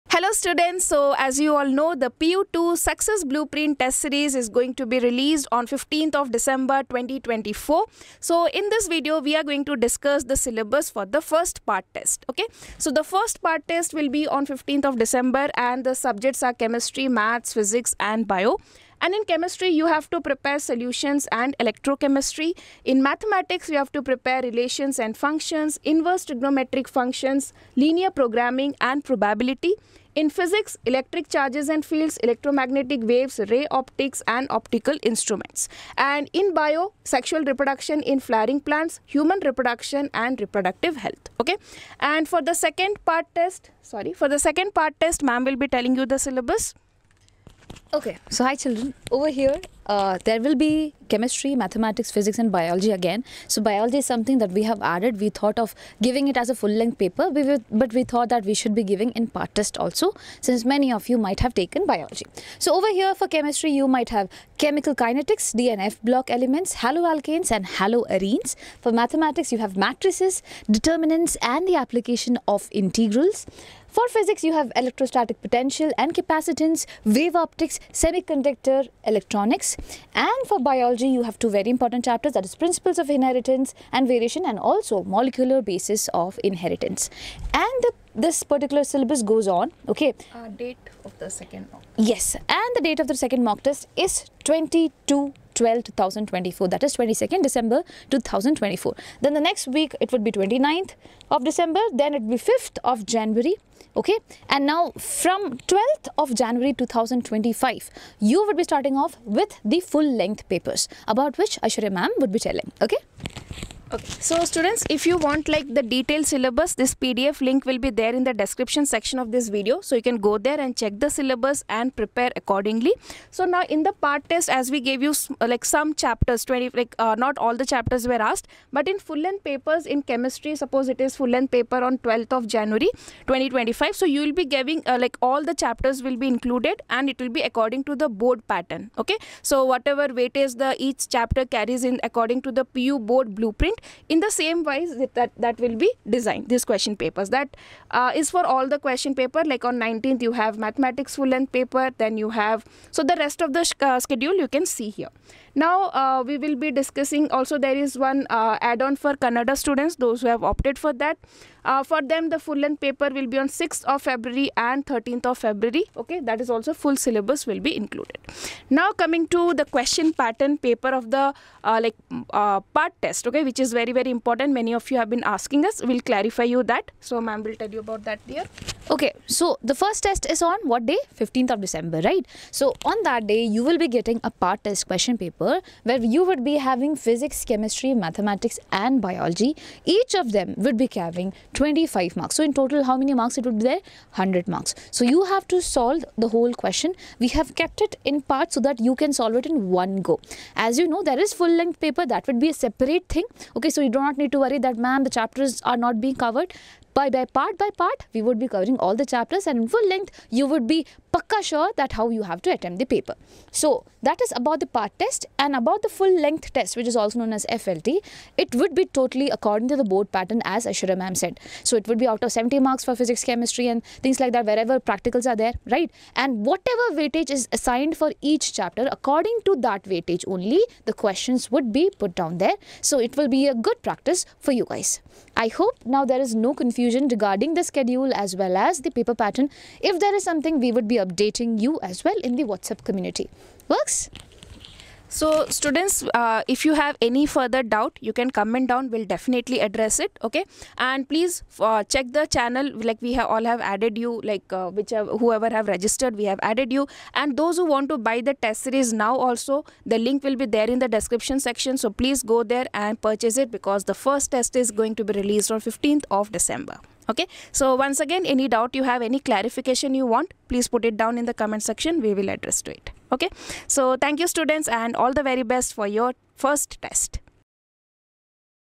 PU 2 success blueprint test series schedule and syllabus
Hello students, In this video Aishwarya ma'am and jasmine ma'am have discussed the syllabus and exam pattern for the PU 2 success blue print test series.